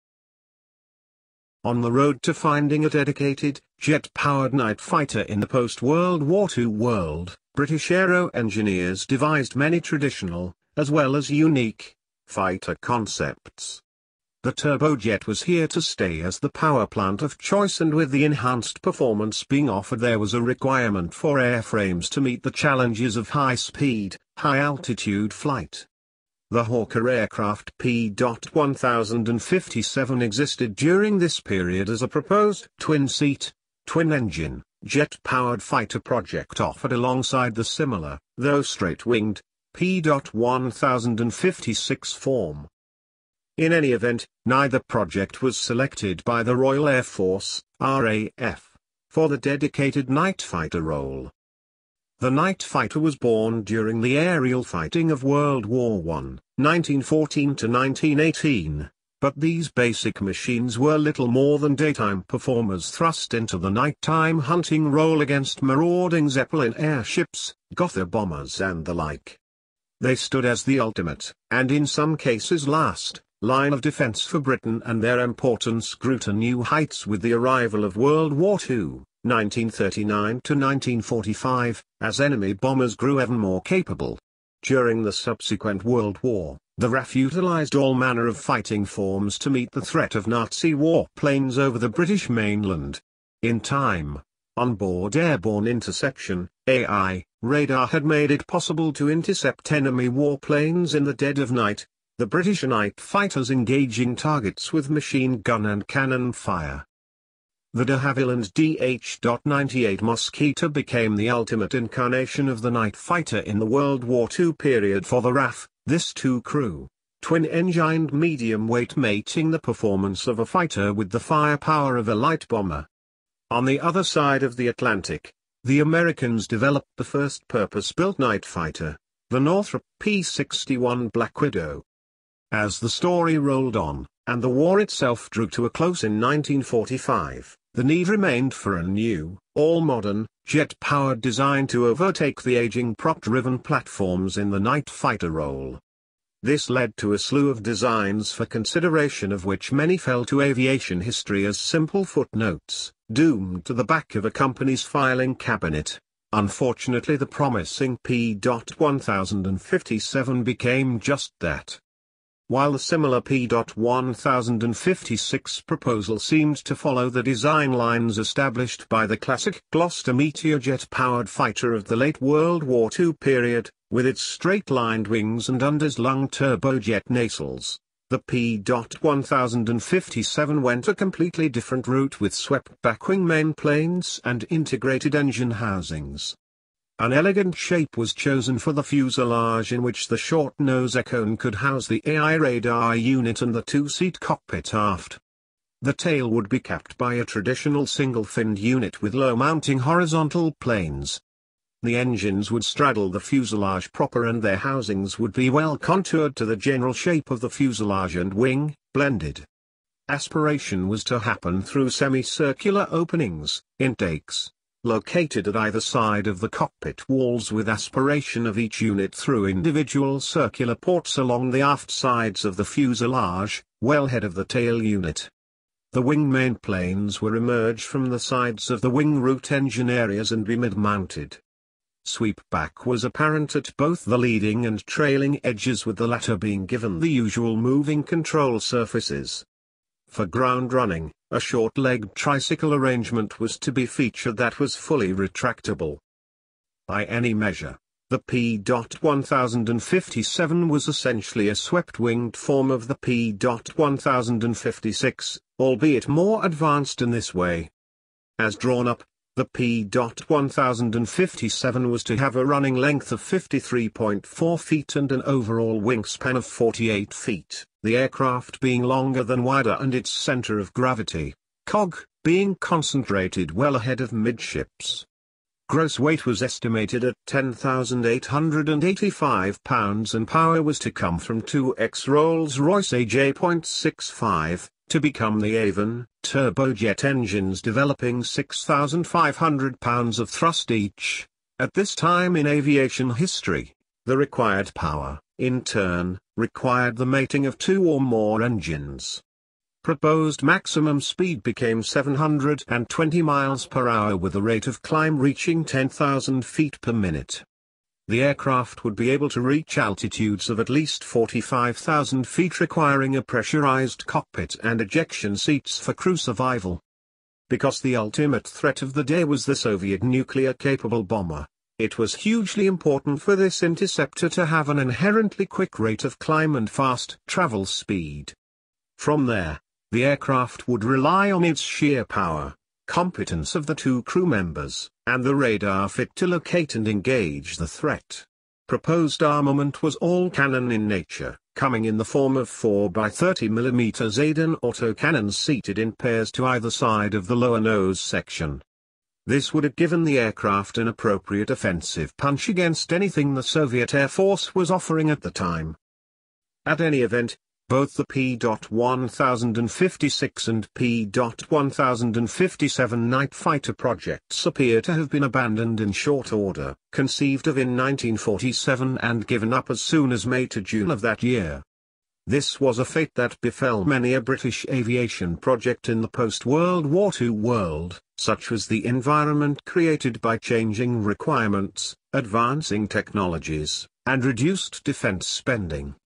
Hawker P1057 Nightfighter Proposal
Details of the short-lived Hawker Aircraft P.1057 nightfighter proposal for the British Royal Air Force during the Cold War period.